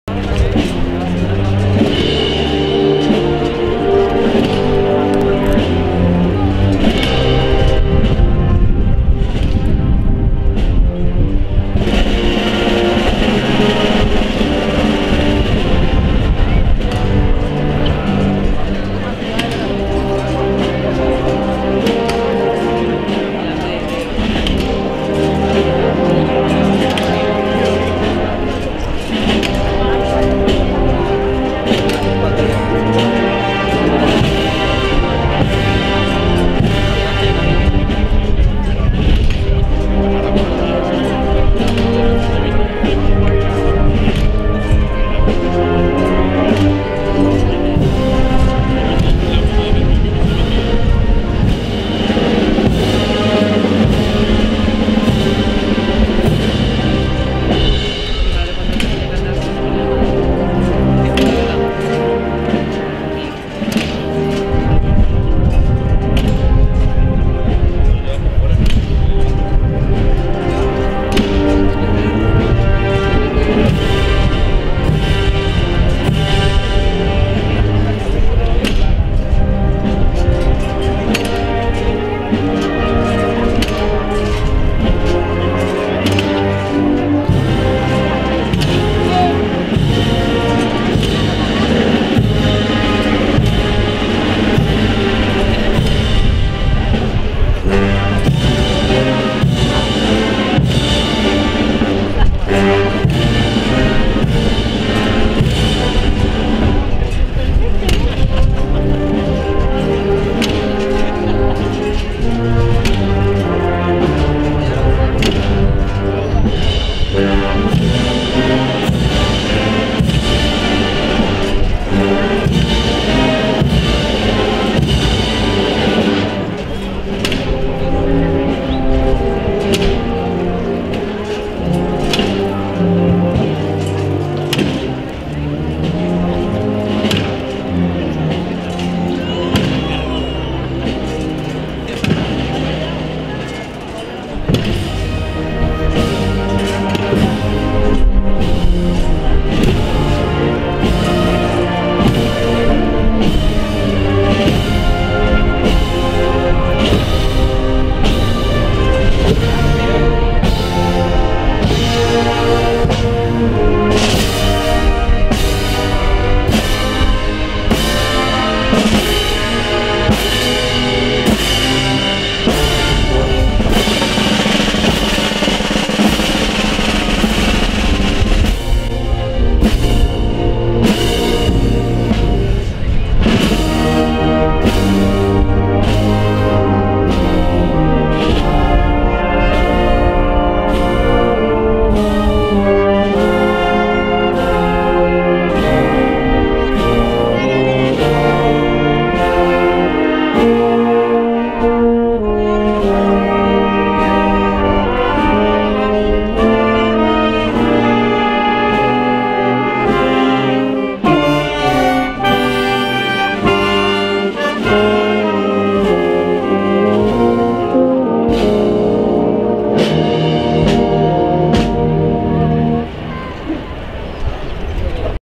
María Stma de los Dolores de Servitas (Banda de Música Maestro Dueñas)
"Orden de Servitas" Viernes de Dolores

             (Cadiz) 7/4/2017